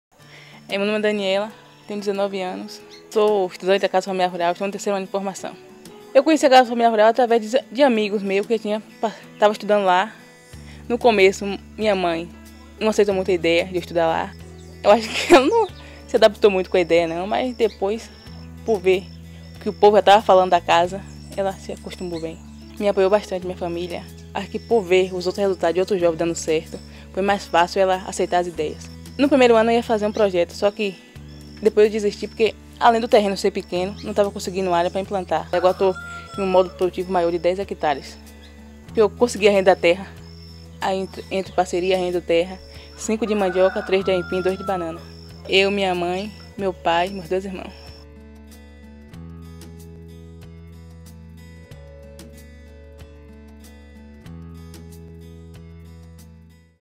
Daniela de Jesus Guedes
Conheça a história da jovem Daniela de Jesus Guedes, ex-aluna da Casa Familiar Rural de Presidente Tancredo Neves (CFR-PTN).